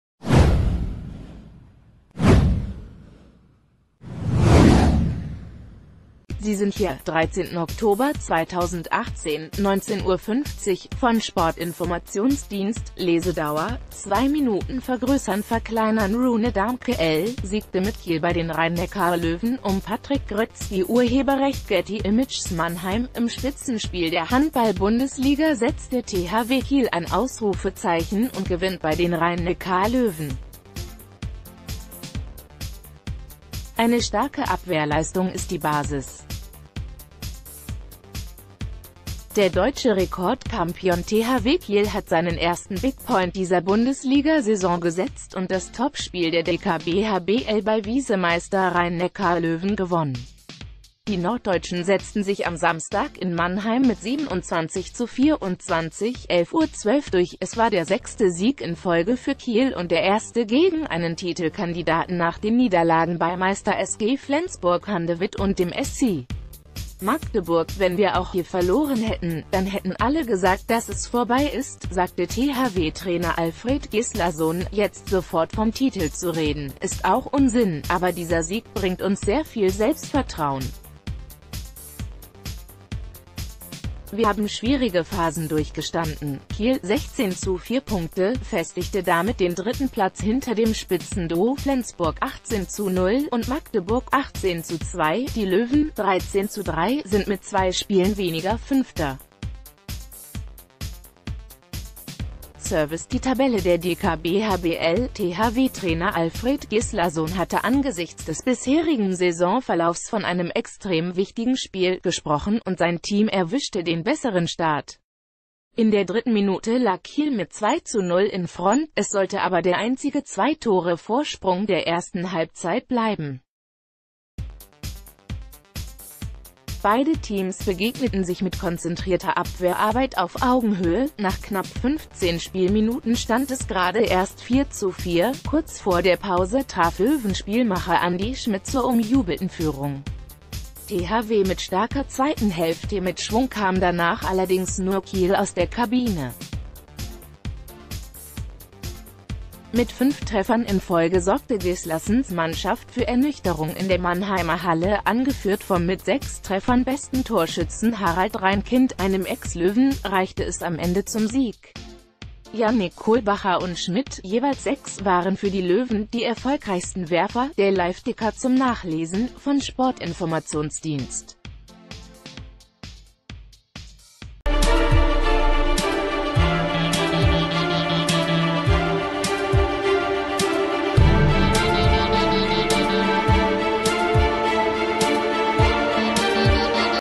HBL: THW Kiel gewinnt Spitzenspiel bei Rhein-Neckar Löwen 27:24
HBL: THW Kiel gewinnt Spitzenspiel bei Rhein-Neckar Löwen 27:24
HBL: THW Kiel gewinnt Spitzenspiel bei Rhein-Neckar Löwen 27:24

Sie sind hier: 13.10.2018 / 19:50 Uhr - von Sportinformationsdienst / Lesedauer: 2 Minuten vergrößernverkleinern Rune Dahmke (l.) siegte mit Kiel bei den Rhein-Neckar Löwen um Patrick Groetzki © Getty Images Mannheim - Im Spitzenspiel der Handball-Bundesliga setzt der THW Kiel ein Ausrufezeichen und gewinnt bei den Rhein-Neckar Löwen. Eine starke Abwehrleistung ist die Basis. Der deutsche Rekordchampion THW Kiel hat seinen ersten Big Point dieser Bundesliga-Saison gesetzt und das Topspiel der DK...